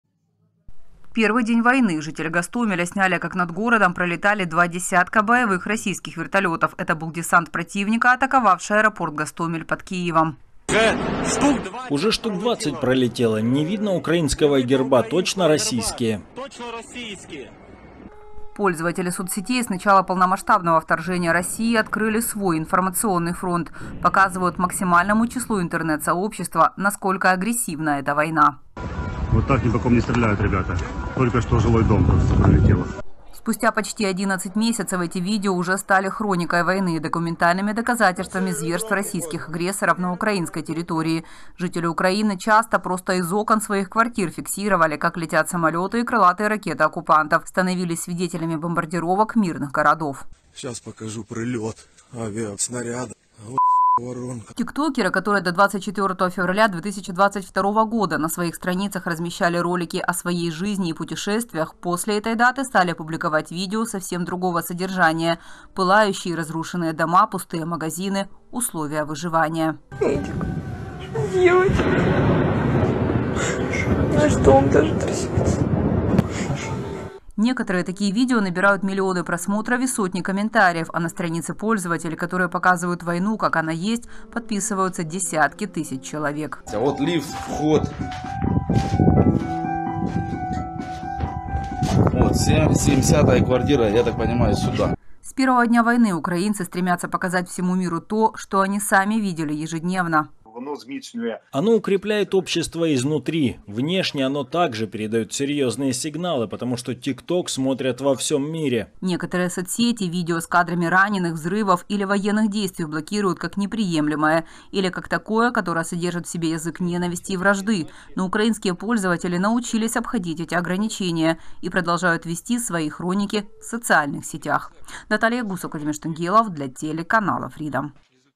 Военный 2022 год в соцсетях. Разбор
Они активно публикуют видео и информацию о войне. Такие ролики набирают миллионы просмотров не только в Украине, но и во всем мире. Украинские блогеры за неполных 11 месяцев войны уже создали ее хронику, поскольку снимали и показывали ключевые события.